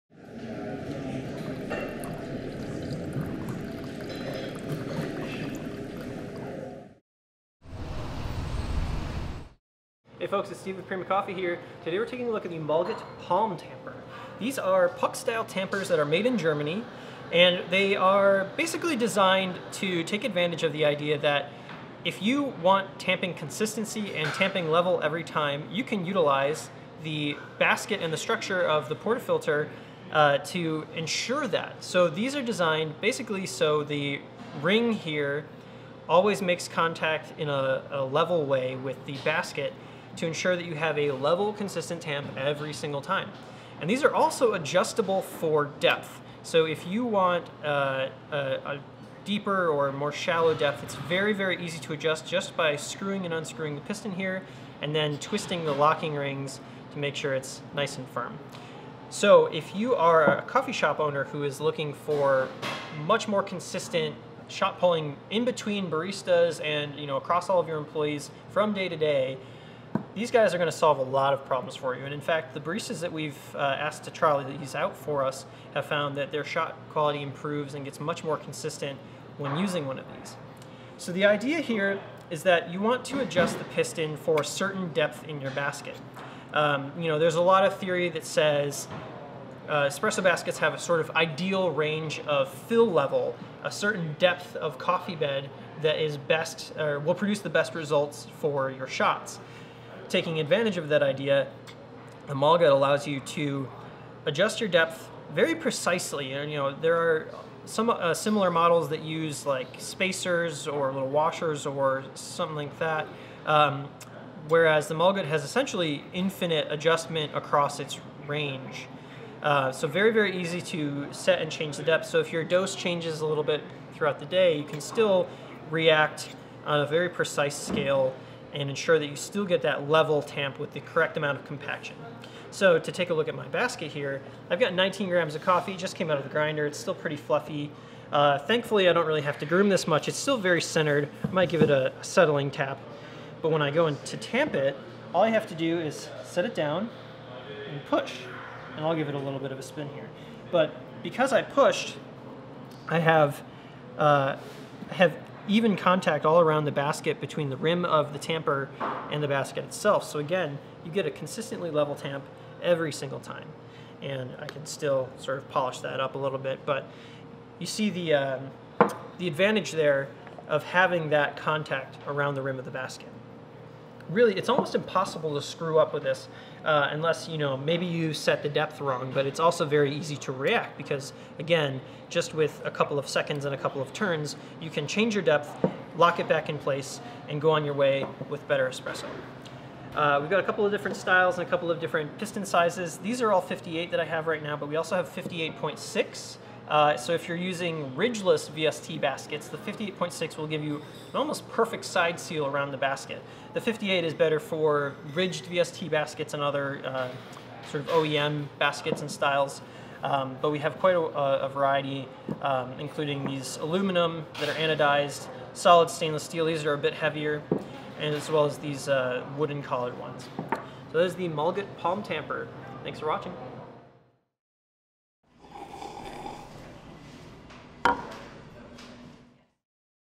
Mahlgut Palm Tamper Overview
Available in three styles: steel, anodized aluminum, and wood, with steel bases in either 58 mm or 58.6 mm diameters. 58 mm bases will fit most standard commercial baskets and ridged VST baskets, 58.6 mm bases are oversized to fit snugly in ridgeless VST baskets. Made in Germany.